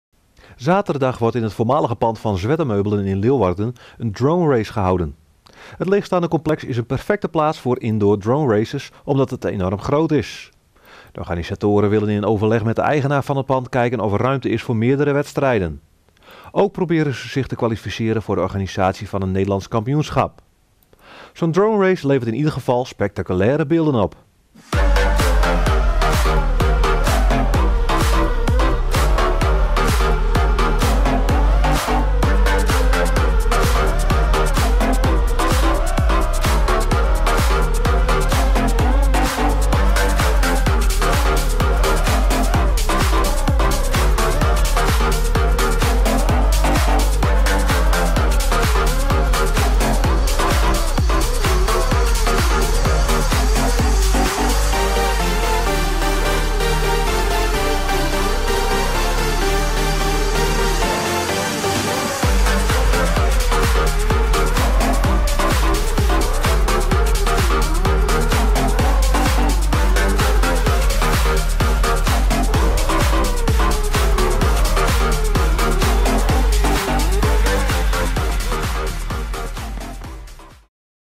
GPTV: Droneraces in Zwettemeubelpand
Zaterdag wordt in het voormalige pand van Zwettemeubelen in Leeuwarden een dronerace gehouden. Het leegstaande complex is een perfecte plaats voor indoor drone races omdat het enorm groot is. De organisatoren willen in overleg met de eigenaar van het pand kijken of er ruimte is voor meerdere wedstrijden. Ook proberen ze zich te kwalificeren voor de organisatie van een NK. Zo'n dronerace levert in ieder geval spectaculaire beelden op.